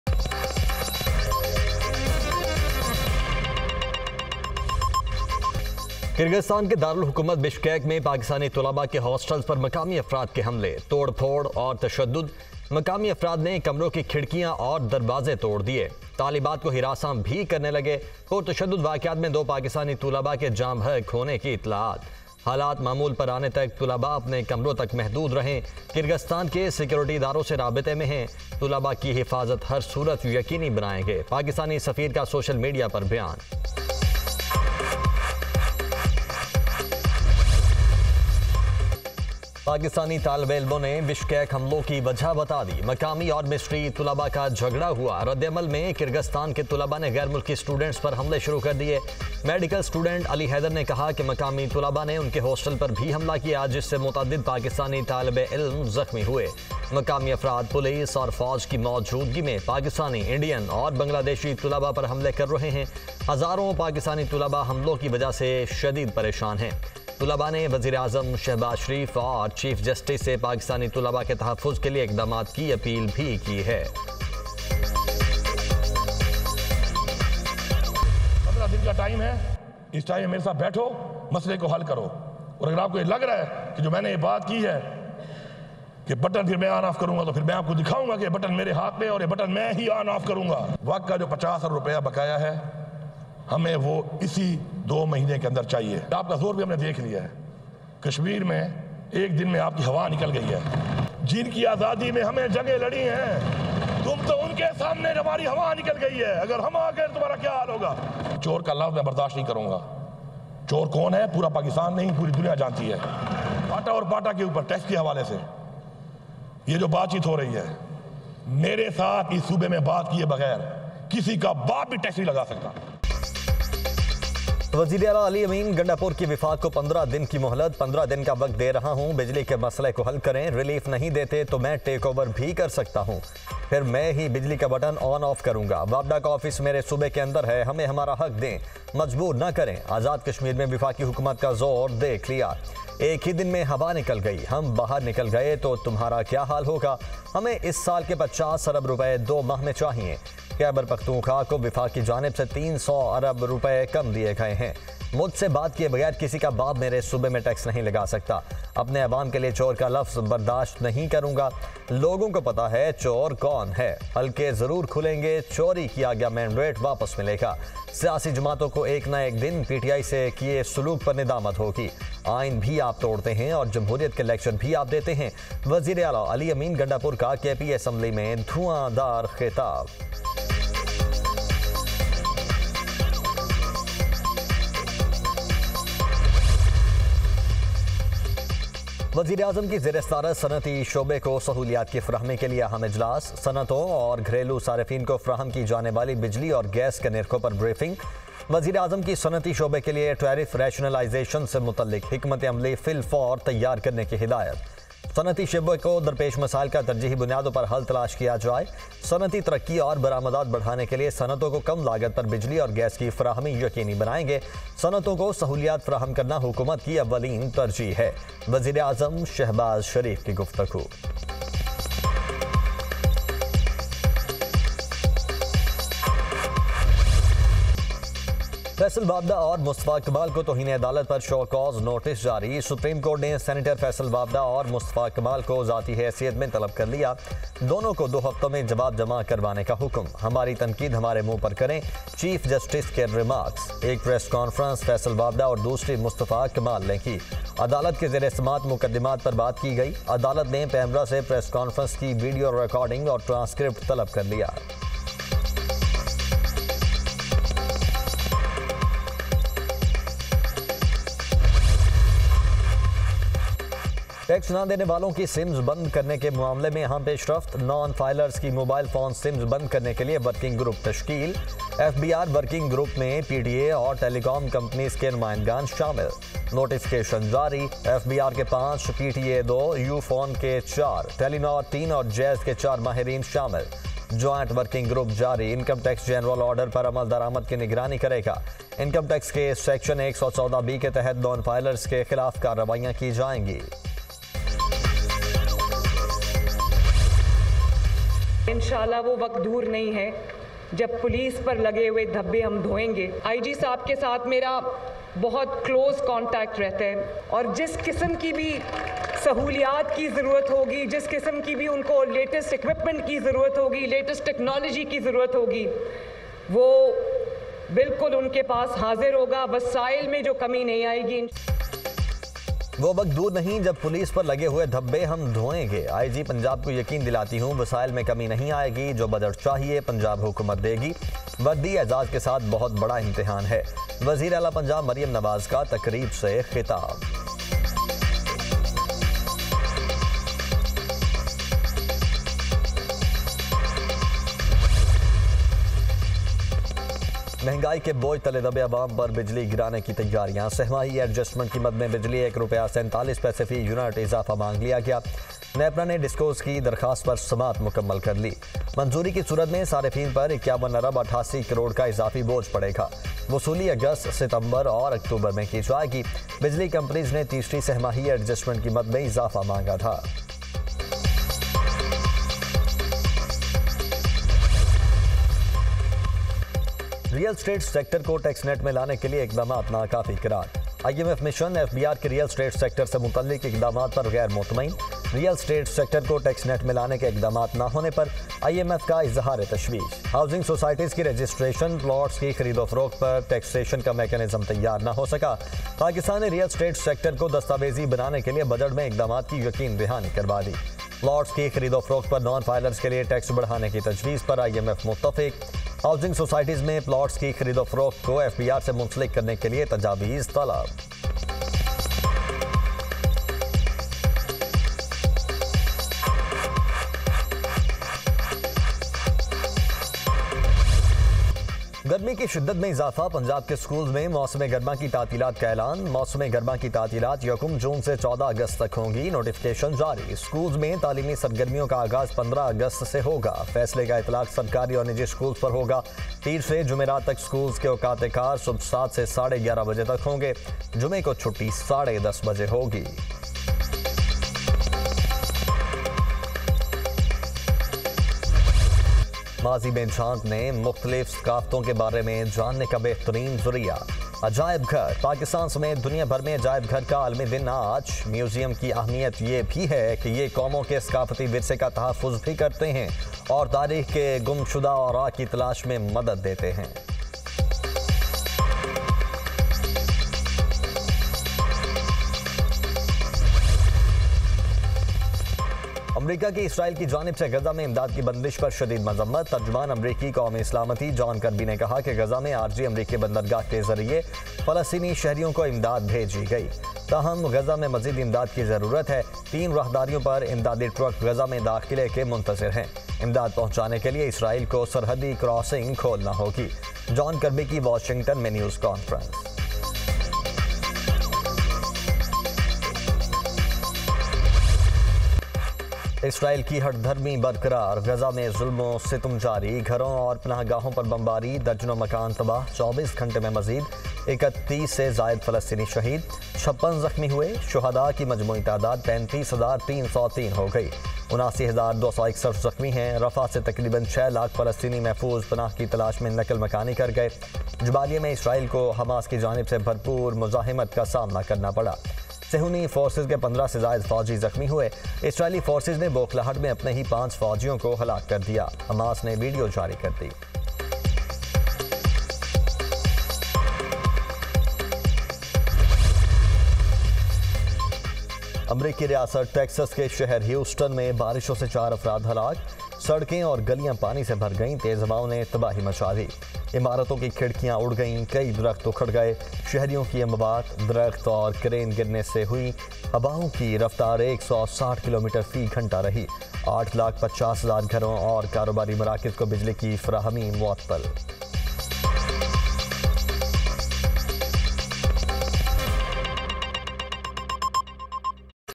Dunya News Headlines 08:00 AM | Serious Situation | Kyrgyzstan Incident | Pakistanis In Danger
Dozens injured in attacks on hostels of Pakistani students in Bishkek

Supreme Court issues show-cause notices to Vawda, Kamal over remarks against judiciary

CM Gandapur gives Centre 15-day ultimatum to fix KP electricity issues

PTI leaders grill Punjab govt over injustice with farmers

Punjab CM Maryam attends Elite Force passing-out parade in police uniform

Govt jacks up power price by Rs1.47 per unit

COAS Asim Munir commends Pakistan hockey team for fine performance

Shiraz bids farewell to vlogging for better cause

Summer vacation for schools in Punjab to start from June 1

 to the official channel of Dunya News. Pakistan’s Top News Network delivering the latest from around the globe.